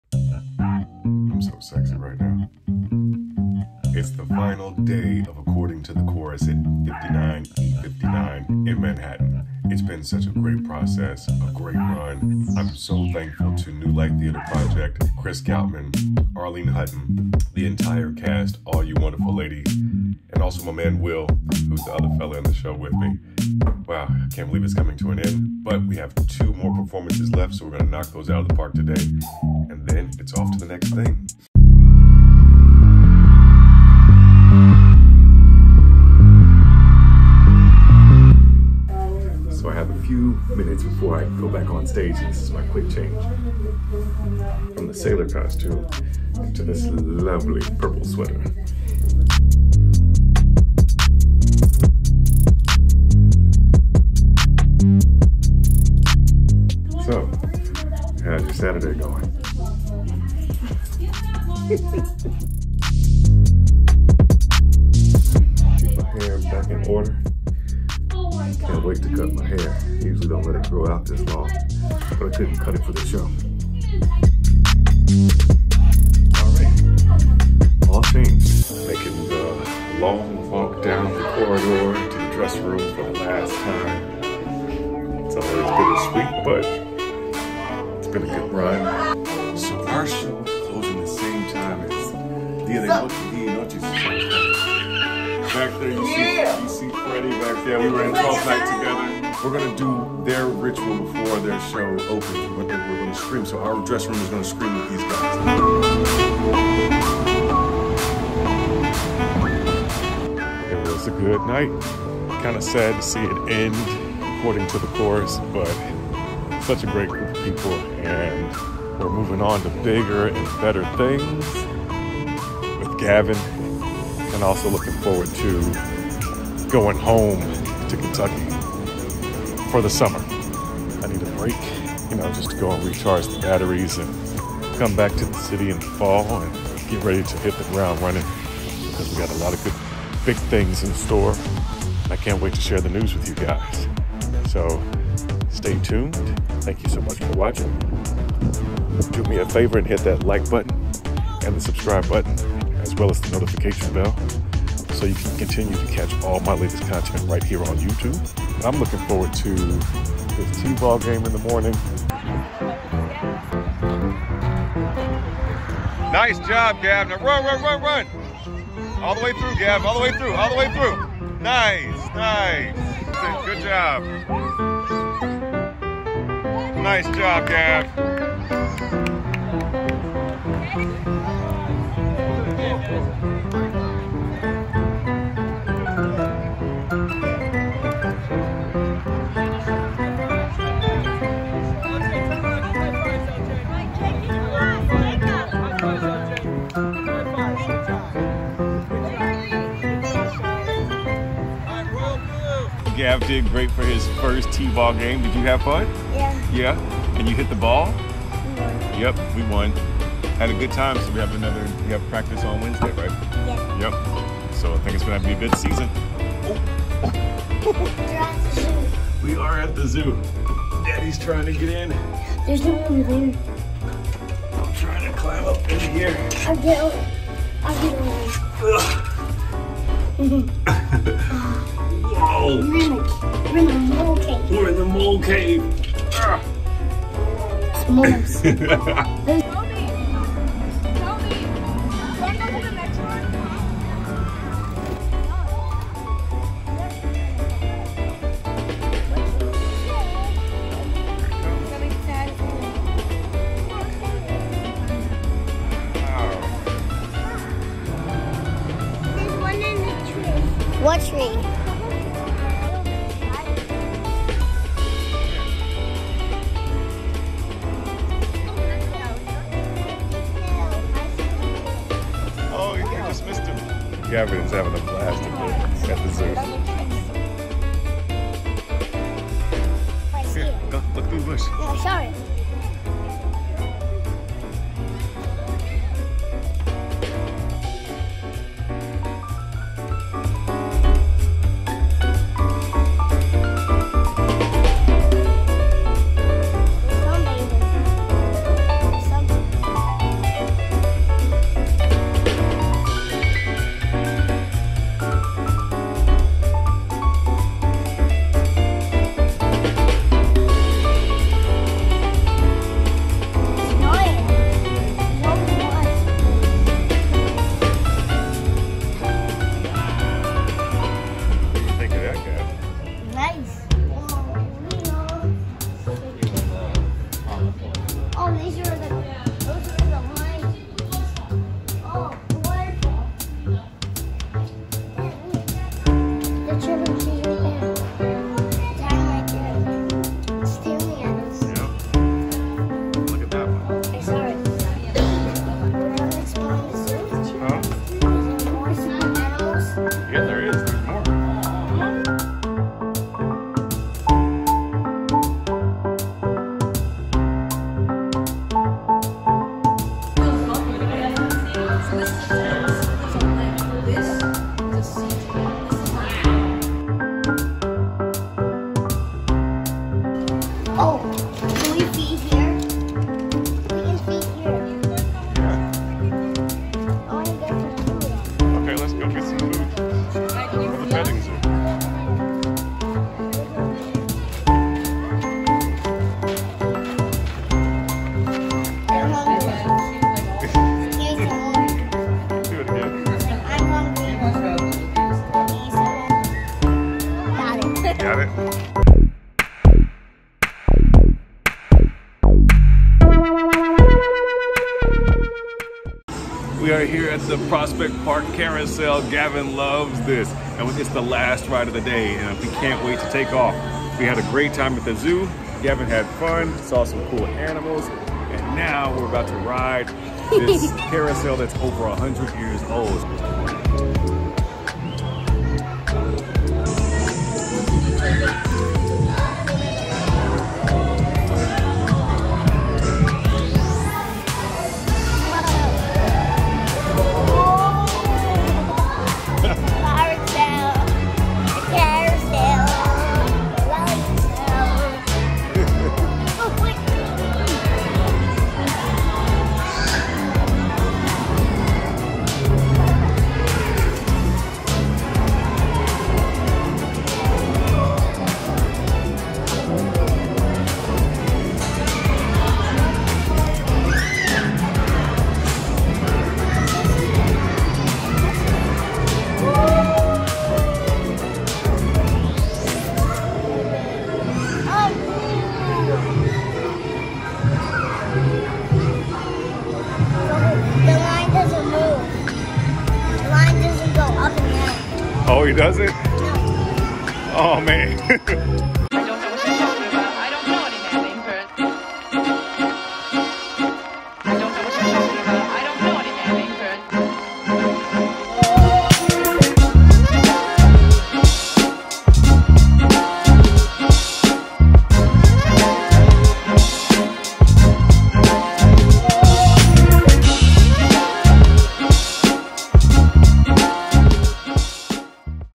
Life of a Working Artist: According to the Prospect Park Zoo
According to the Chorus comes to a close and it’s time for some quality father and son time. The first T-ball game of the year and a trip to the zoo is in store for the Jones Boys.